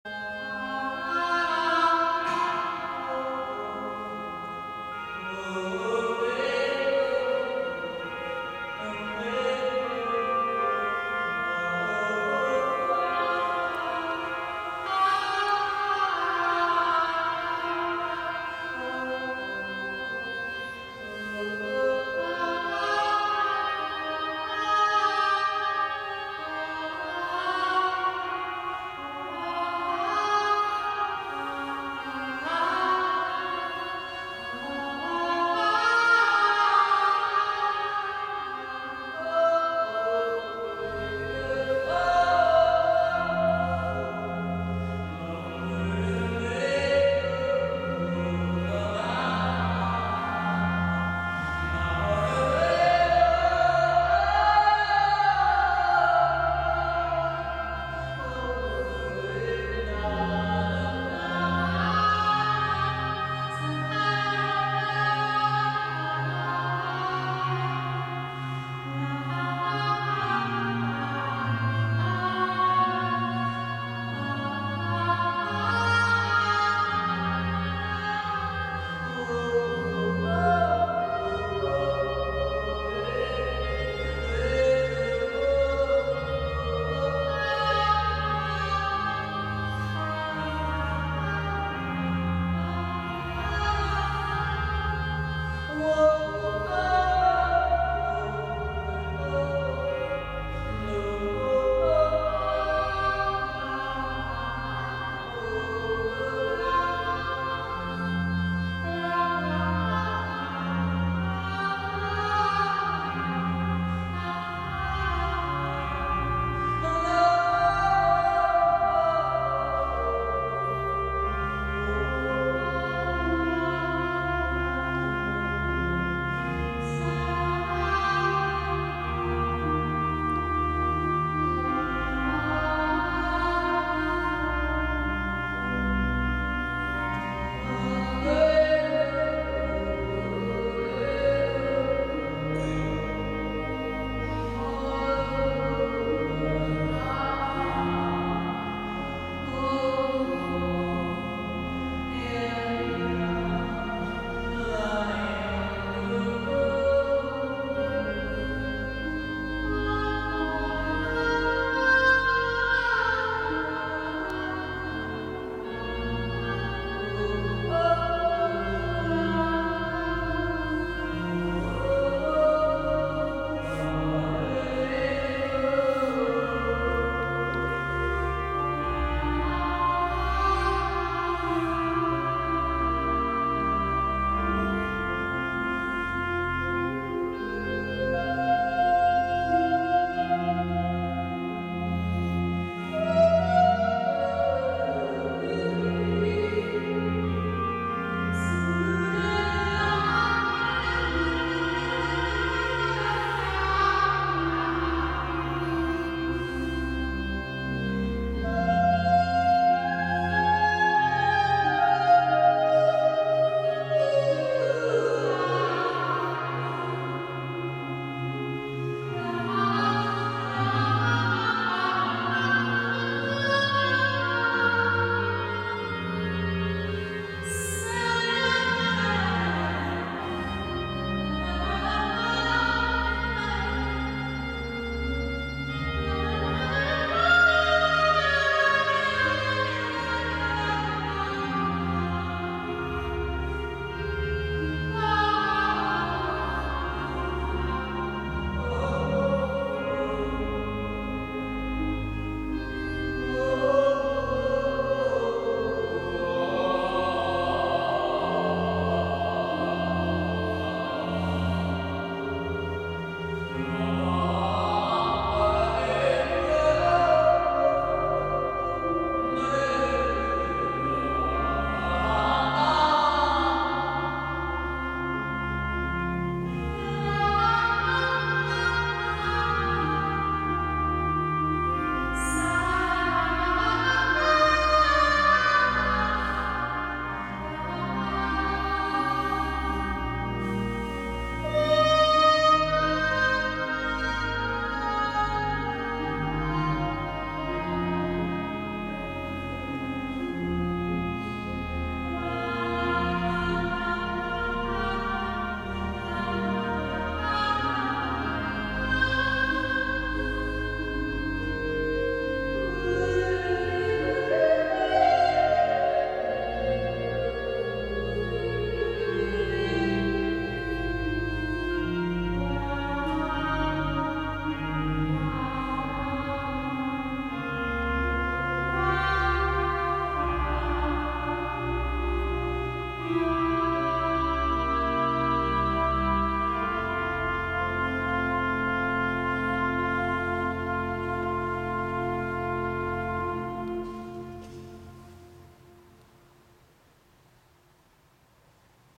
Improviser meets Bach. 12. St.-Josef-Kirche Moers, Michael Schiefel + Boris Berns
Geistliche Konzerte an St. Josef
Orgelwerke von J.S.Bach, Gregorianik und Improvisation
Michael Schiefel, Improviser in Residence, Gesang
Boris Berns, Orgel & Gesang
St. Josef, Moers am Kastellplatz